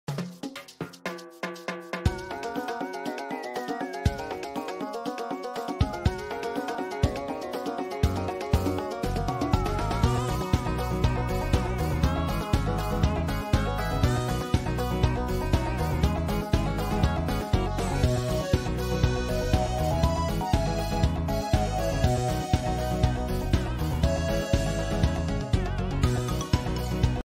Elimination Obstacle Course (10e)
14 stickfigures get challenged to one of their hardest obstacles yet!